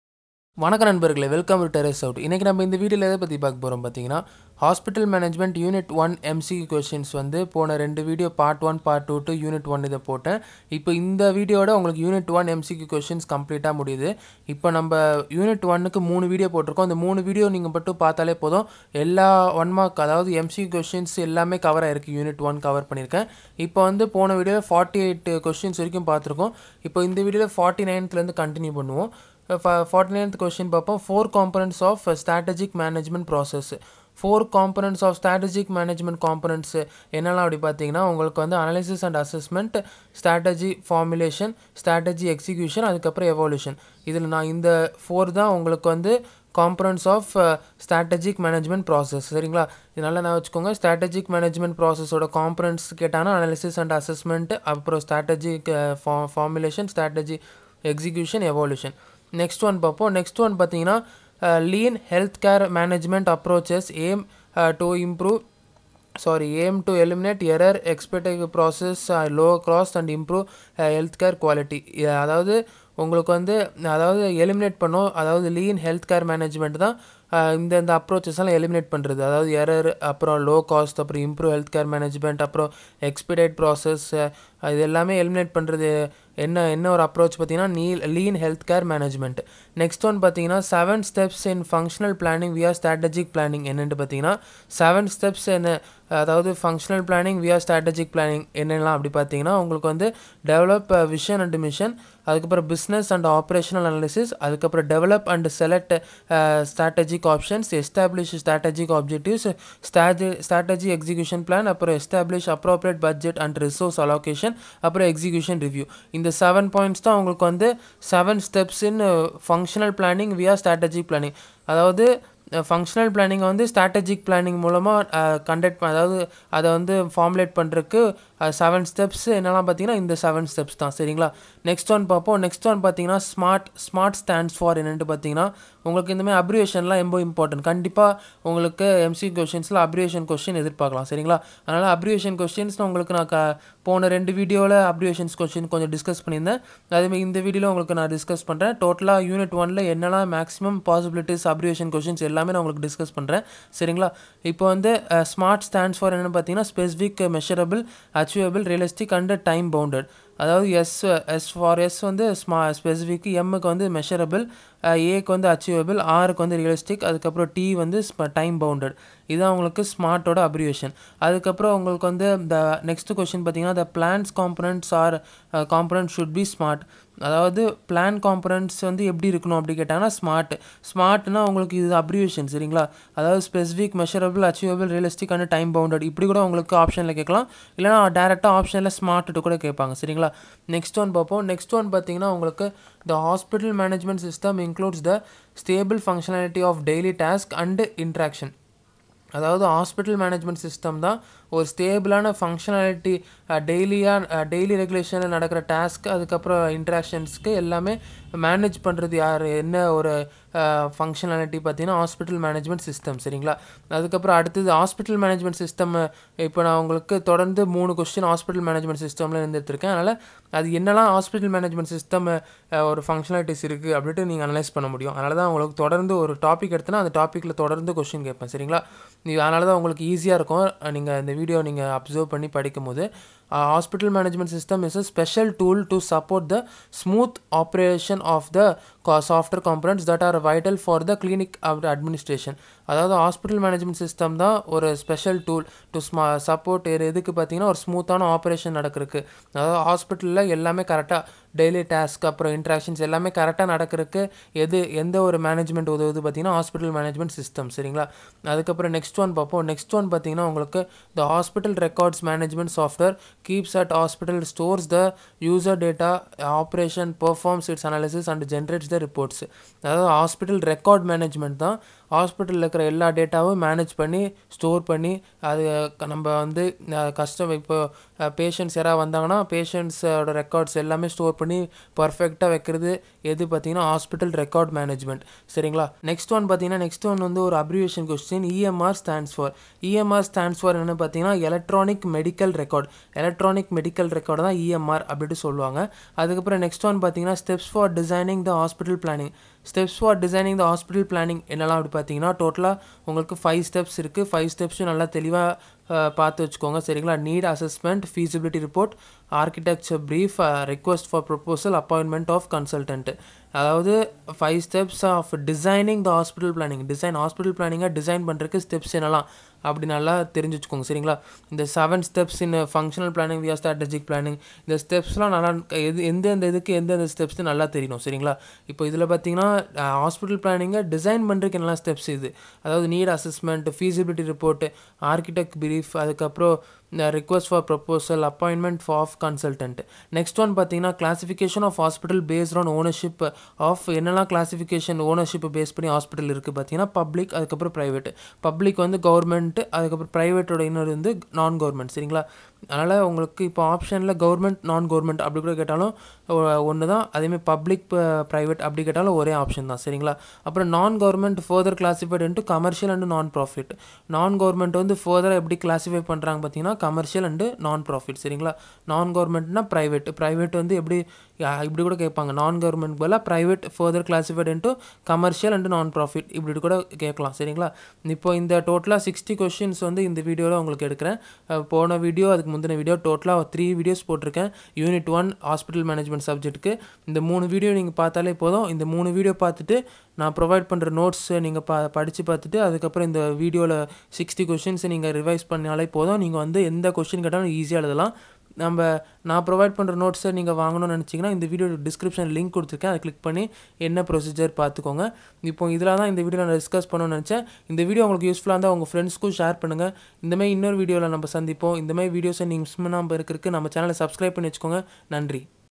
Hospital Management | Anna University | MCQ | Unit-1 | Tamil
From This Video You Will Be Able To Understand The MCQ Anna University Questions Important Unit-1 Part-3